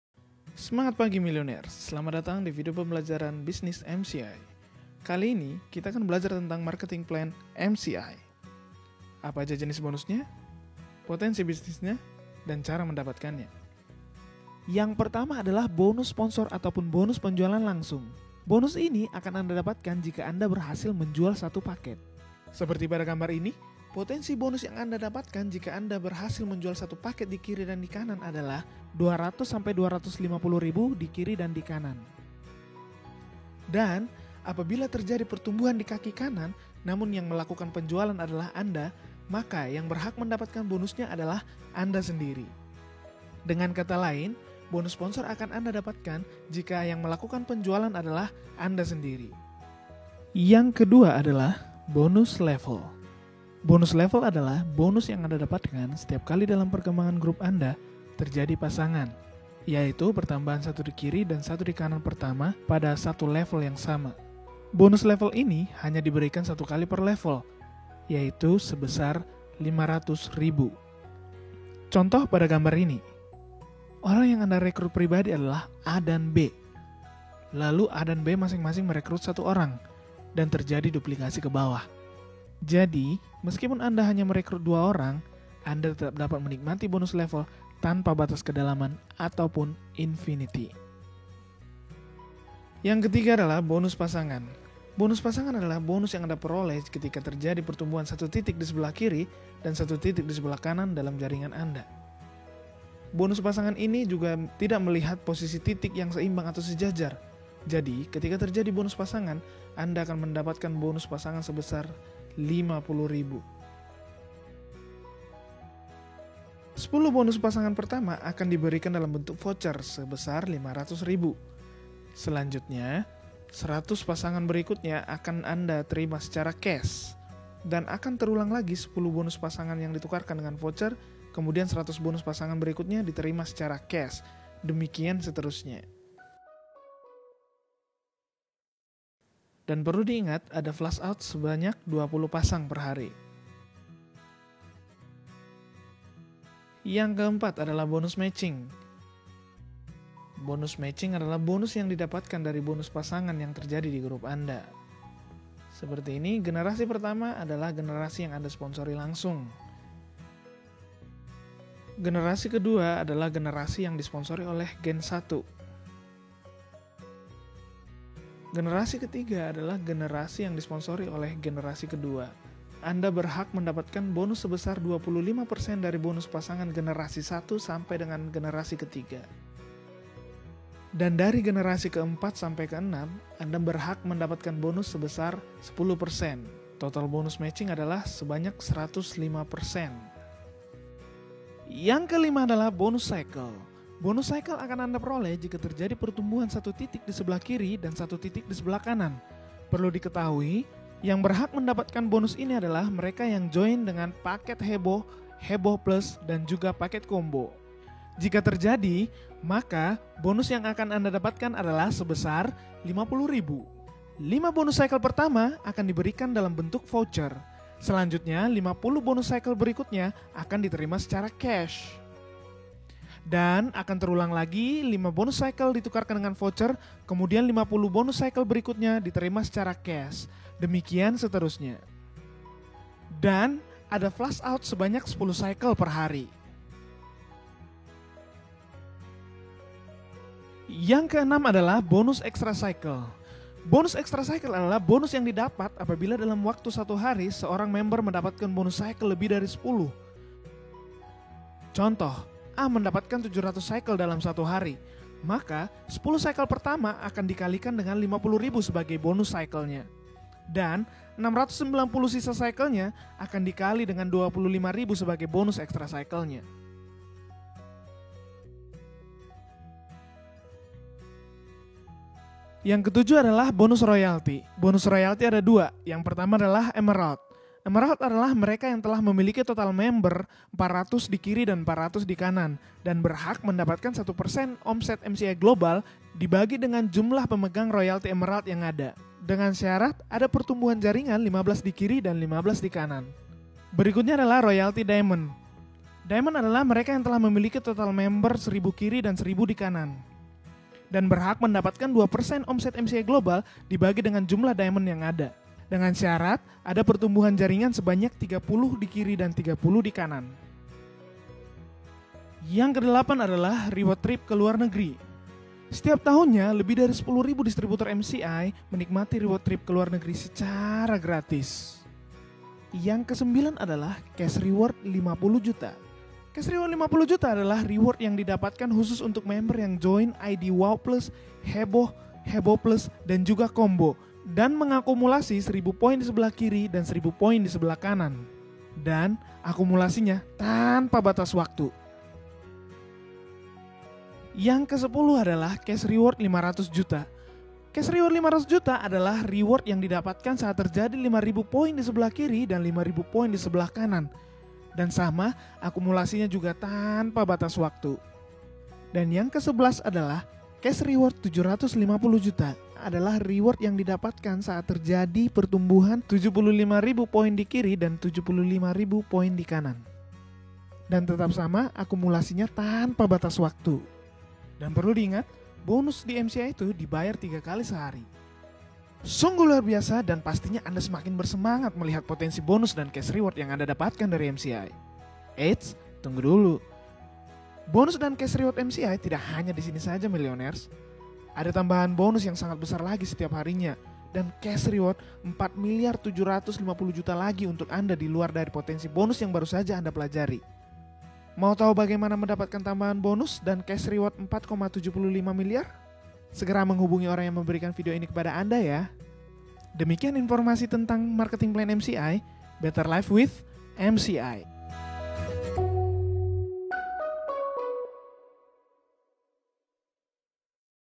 Marketing plan |MCI
Semangat pagi Millionaires!

Pernahkah anda ditanyakan oleh rekan atau calon rekan Anda,

"Bagaimana perhitungan marketing plan di MCI?"

"Berapakah potensi penghasilan yang bisa saya dapatkan ketika bergabung dan menjalankan bisnis di MCI?"

"Berapa banyak reward-reward yang bakal mereka dapatkan ketika bergabung dan menjalankan bisnis di MCI?"

Untuk menjawab semua pertanyaan tersebut, marilah kita saksikan video di bawah ini agar anda dan rekan anda semakin sadar betapa mudah dan besarnya penghasilan yang bisa Anda dapatkan melalui Bisnis MCI.

Pastikan Anda segera share video ini ke rekan-rekan dan calon rekan Anda.

Better Life with MCI.